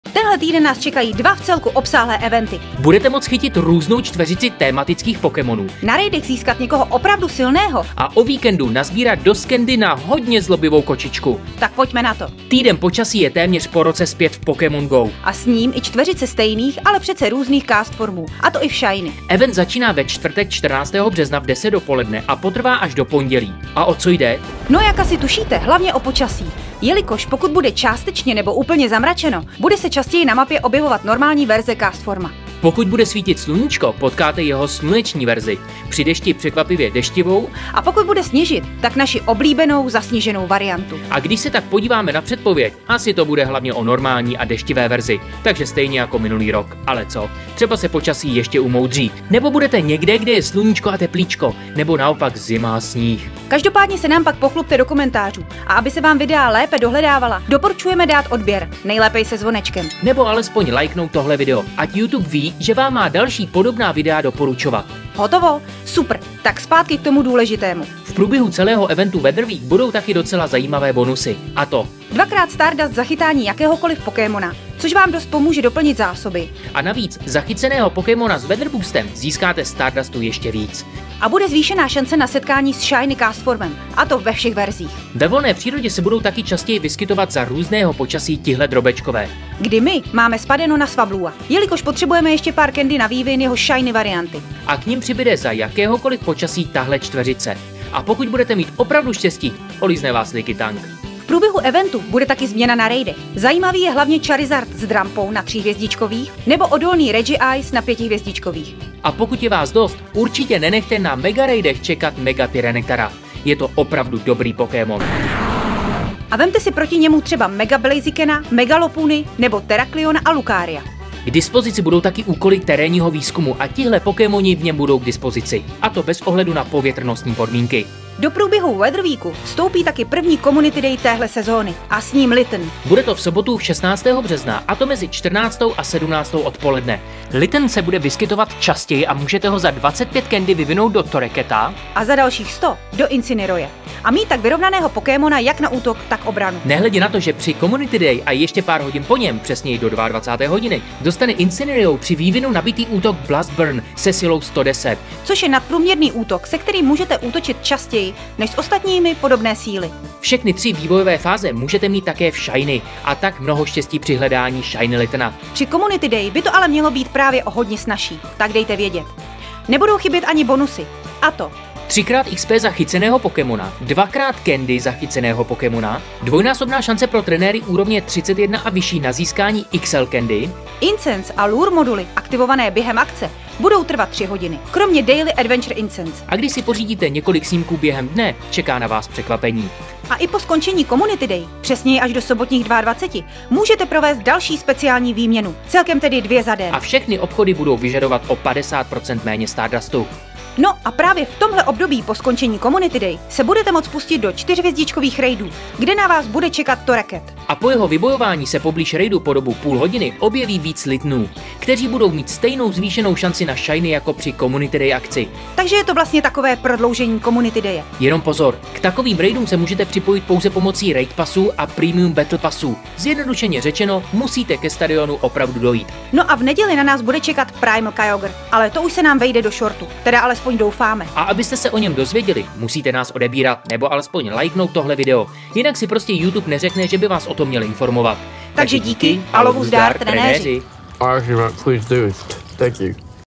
PŘICHÁZÍ WEATHER WEEK A COMMUNITY DAY! ŠANCE PRO VÁS JAK CHYTIT SKVĚLÉ POKÉMONY!
Tenhle týden nás čekají dva vcelku obsáhlé eventy. Budete moct chytit různou čtvrřici tématických pokémonů, na raidech získat někoho opravdu silného a o víkendu nasbírat dost candy na hodně zlobivou kočičku. Tak pojďme na to.

Trainers code v Pokémon GO:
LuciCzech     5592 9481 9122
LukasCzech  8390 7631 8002

Díky, že nás sledujete. Budeme rádi pokud video olikujete, je to pro nás skvělá zpětná vazba. Vaši Pixličci :-).